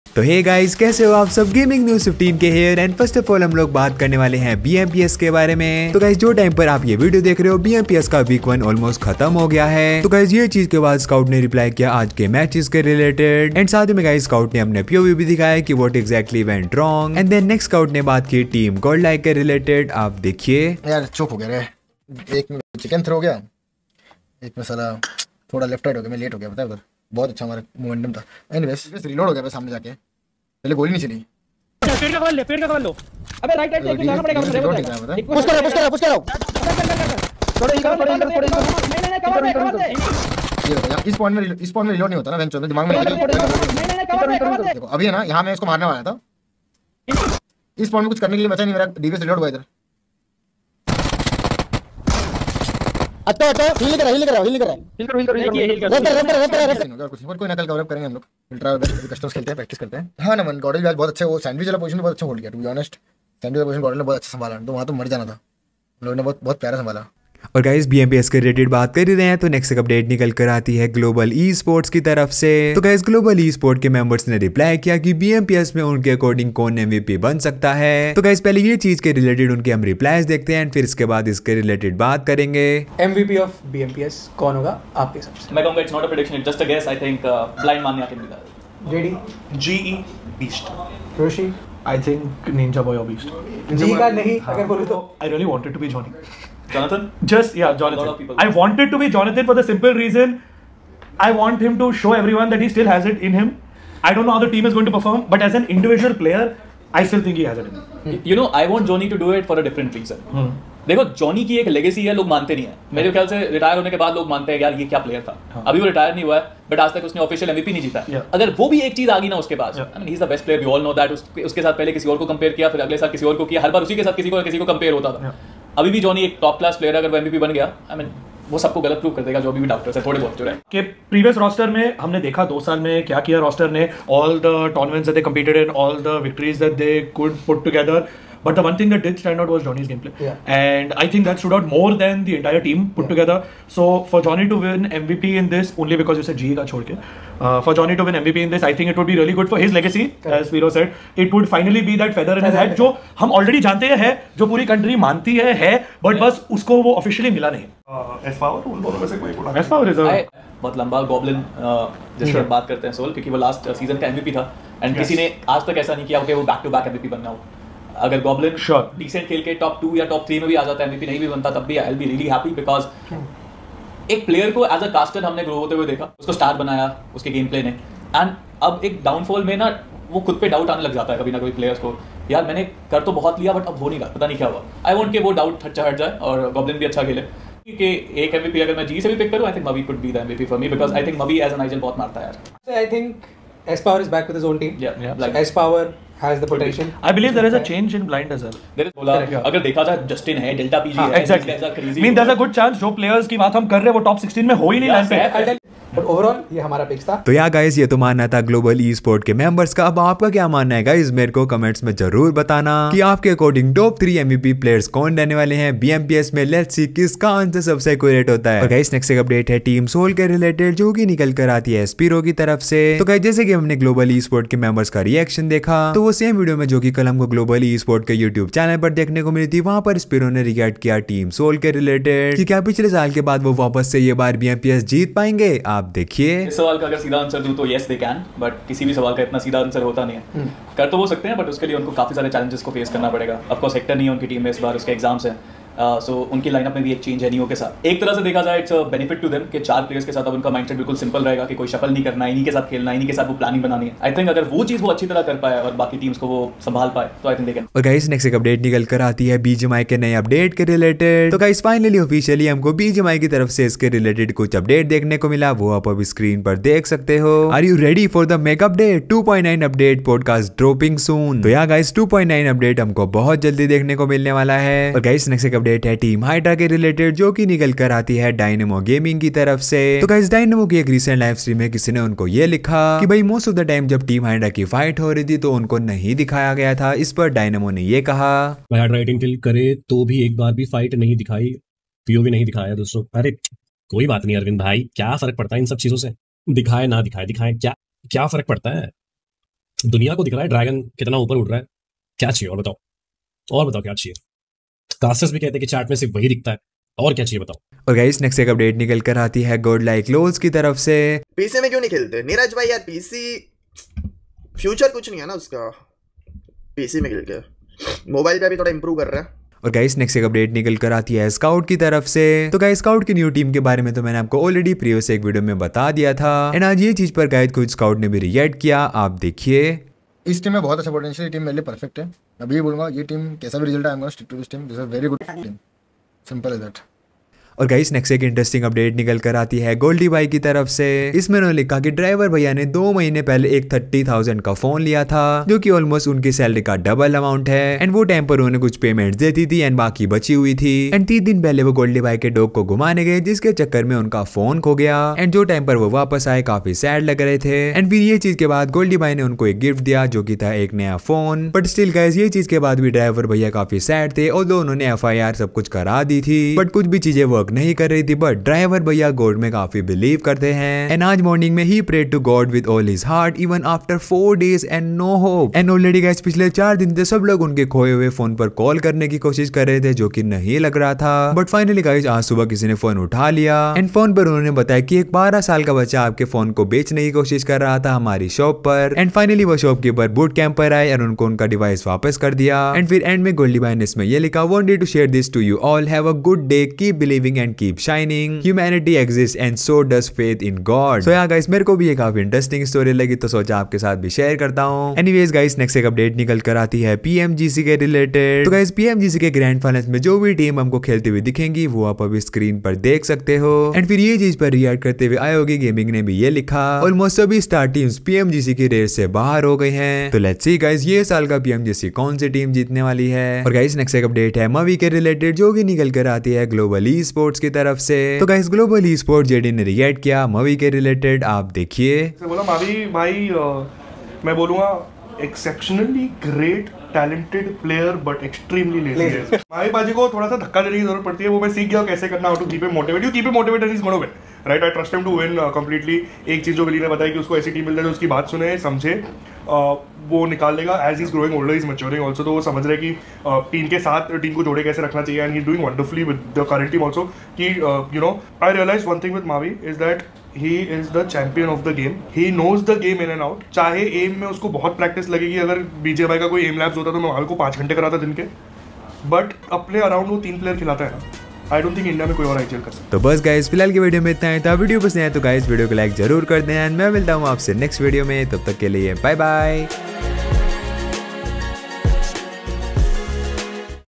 Ab MVP Kon?..😯 SCOUT on GODLIKE🔥 All Reply to JONATHAN, SouL Goblin, Goldy Bhai,Dynamo,Mavi😳 GodLike
- Thanking you for Lovely Support

00:00 - Shocking News for Everyone
00:09 - Scout Reaction, What Went Wrong
00:30 - Scout Showing Proofs and Explained this
00:59 - GodLike Reaction by Scout
01:12 - Ab MVP Kon? All Reaction
01:36 - Jonathan, All Reply to JONATHAN
02:40 - SouL Goblin, All Reply
03:39 - Can Team SouL Win BMPS 2023 Tournament?
04:18 - BGMI Big Announcement
04:35 - Dynamo Reply on Hydra Team, Statement?
05:08 - GodLike Lolzzz Reply
05:20 - Scout First Reaction on New Team
05:38 - Goldy Bhai Story, Full Emotional Story
06:45 - Grand Finals Team Revealed
07:03 - Why Mavi is the Only Player in India?